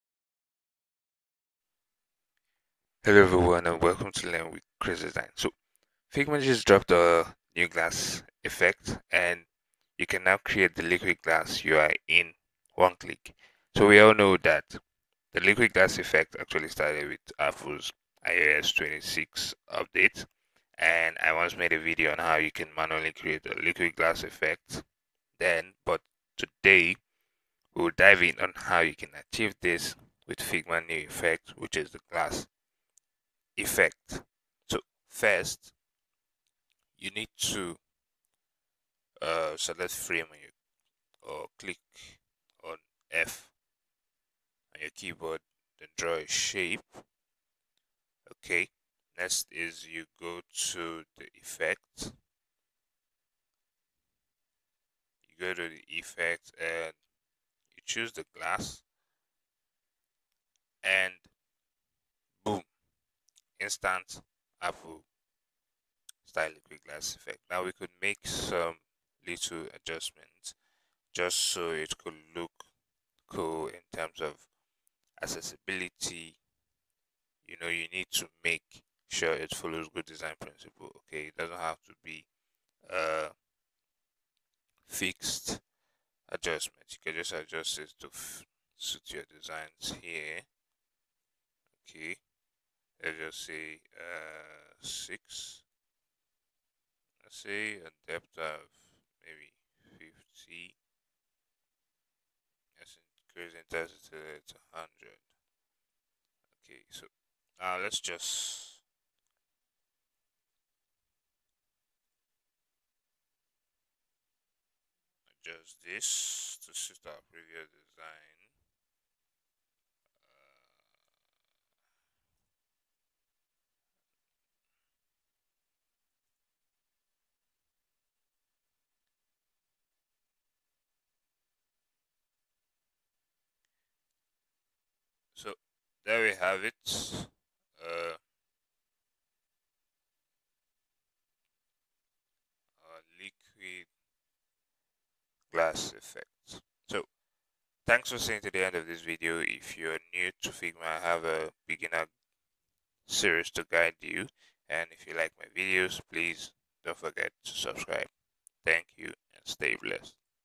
This New Figma Glass Effect is Crazy! (Liquid Glass in 1 Click)
✨ Figma just dropped a new **Glass Effect**, and you can now create **Liquid Glass UI** in ONE CLICK!

➡️ Works only on frames, so grab the frame tool and try it.
🔥 Perfect for Apple iOS-style designs & modern UI cards.